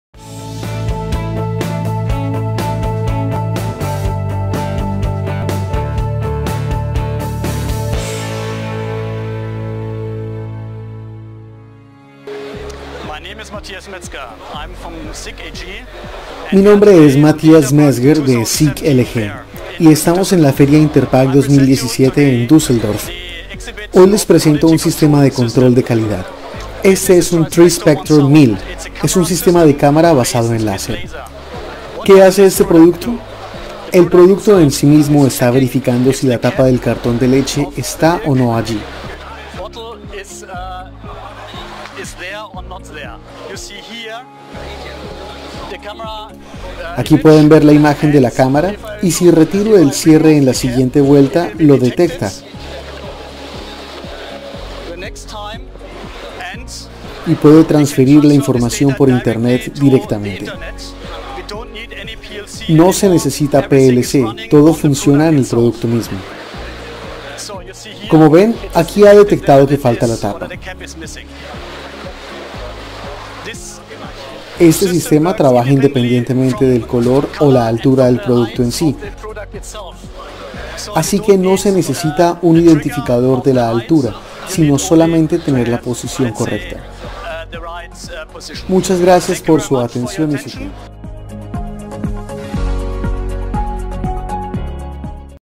SICK en interpack 2017 Parte II - Cubrimiento especial de El Empaque+Conversión
Matthias Mezger, representante de la compañía, presentó para El Empaque+Conversión el sistema de control de calidad TriSpector 1000 que se está exhibiendo en el Messe Düsseldorf, en el marco de la mega-feria interpack 2017.